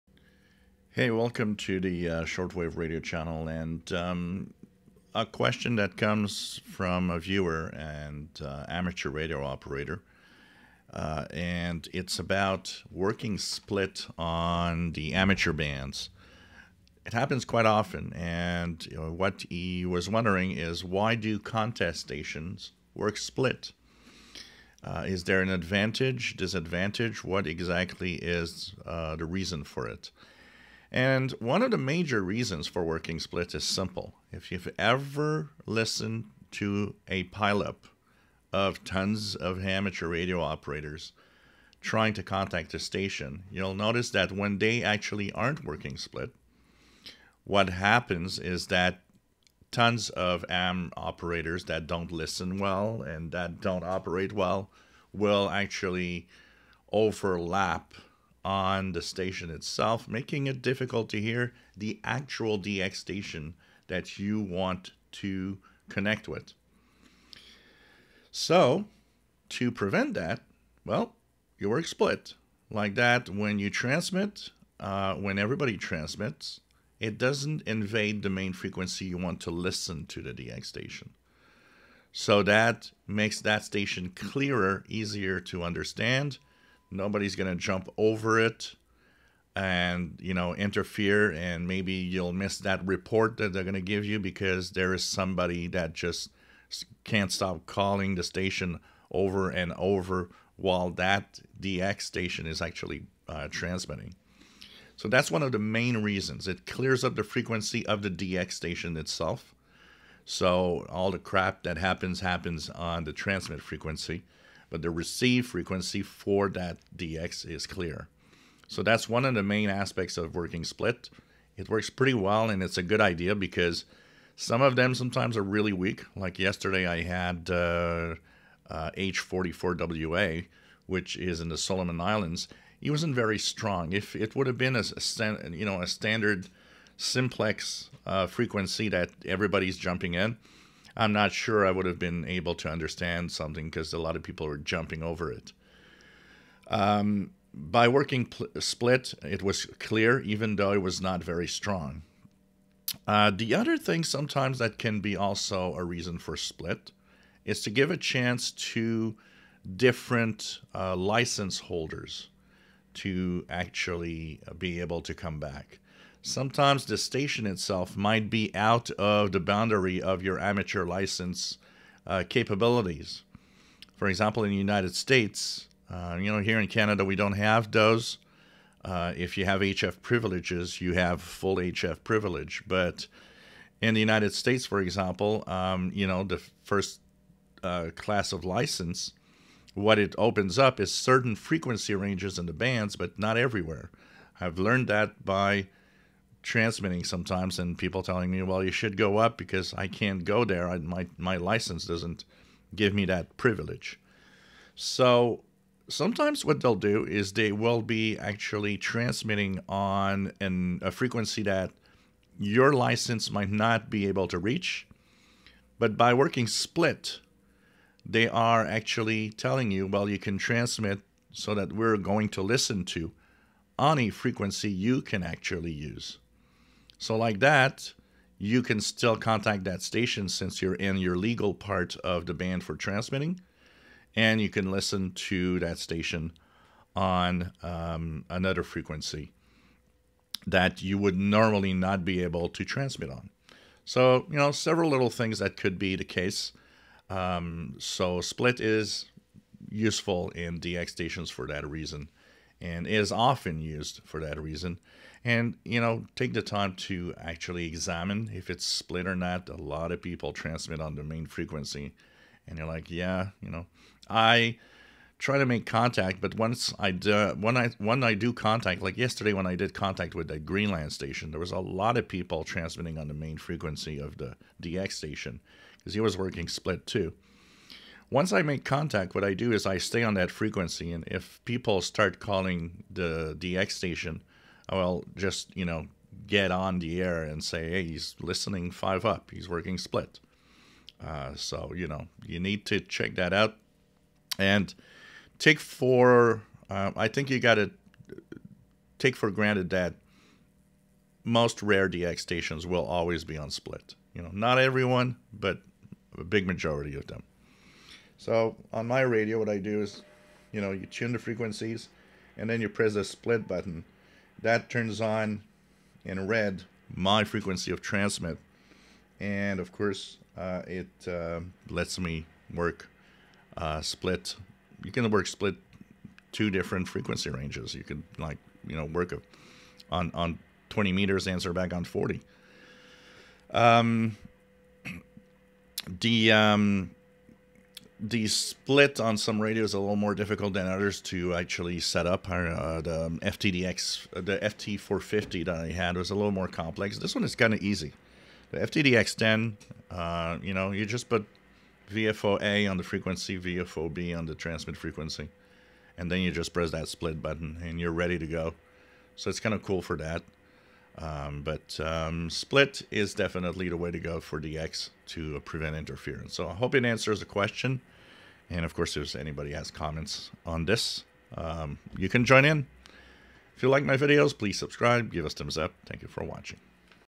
Amateur radio question Why do contest stations work split using two different frequencies
Viewer question about working split on amateur radio